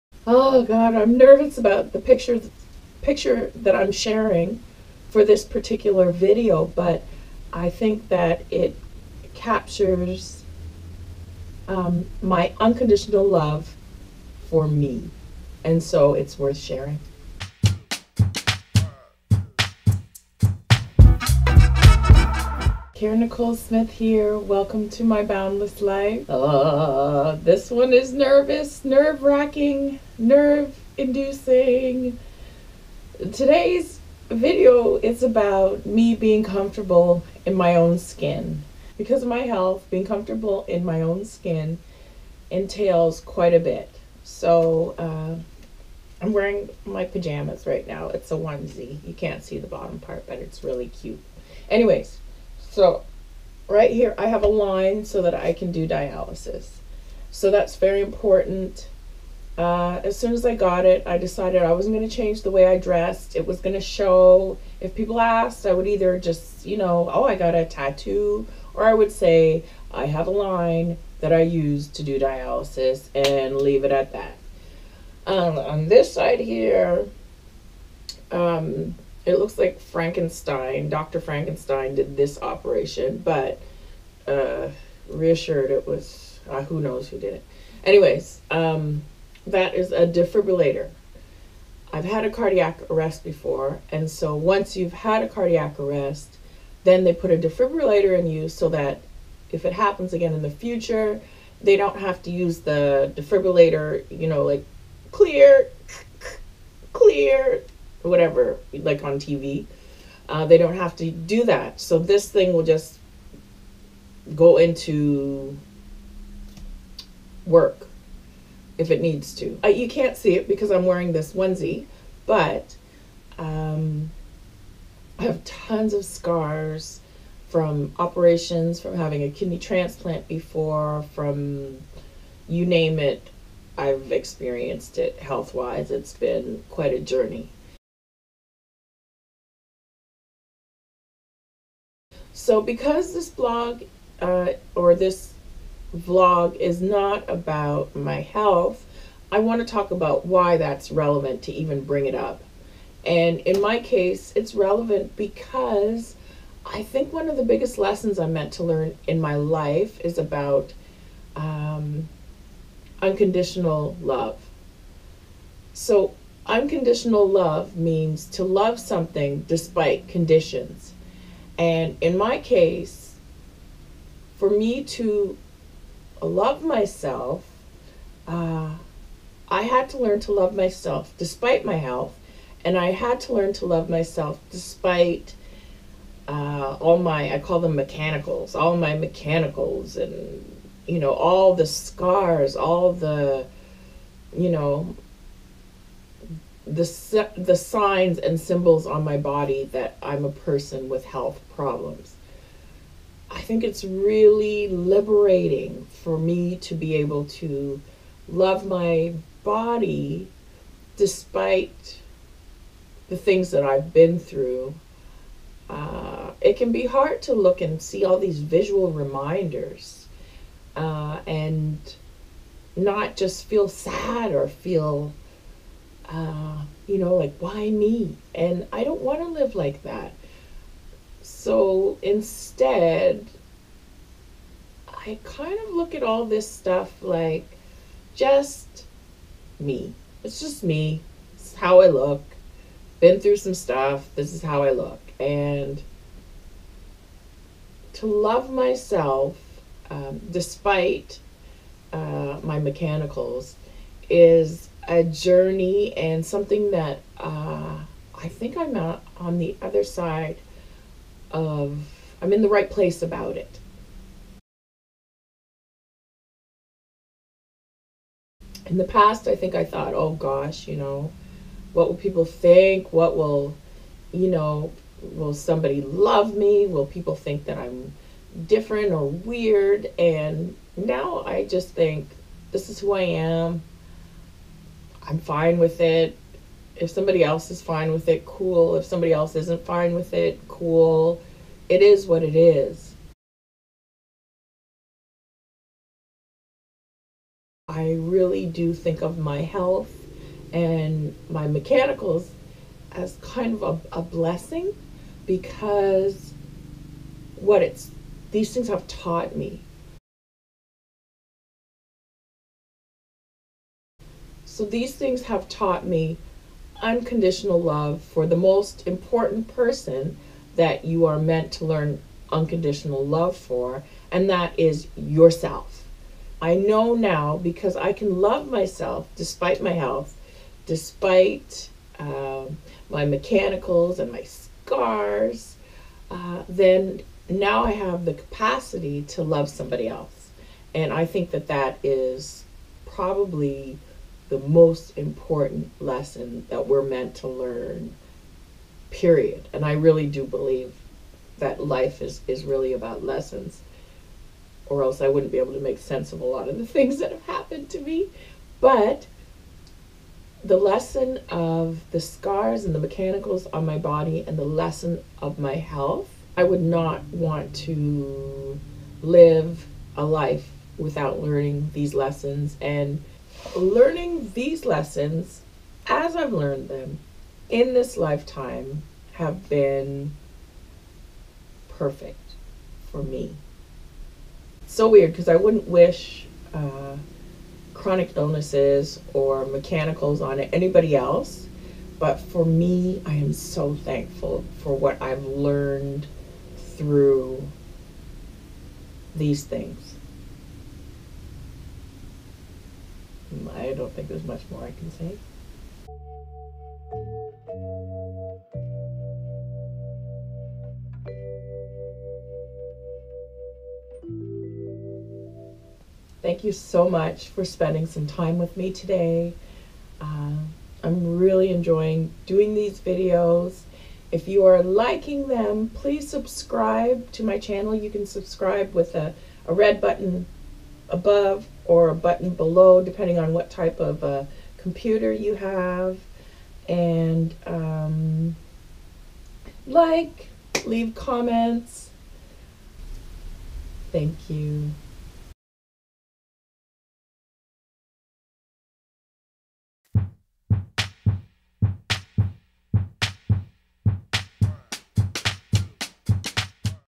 Skin I'm In - Karen Nicole Smith
Todays video is about making peace with my health and my medical devices. Most importantly in the skin I'm in . . . as it relates to my boundlesslife.